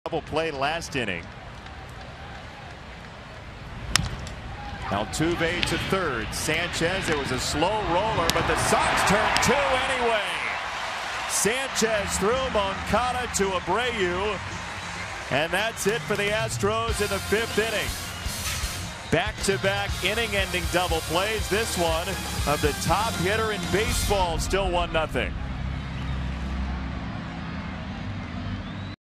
HOU@CWS: Sanchez starts a smooth 5-4-3 DP
Yolmer Sanchez picks up a grounder and fires to second, starting a 5-4-3 double play in the top of the 5th inning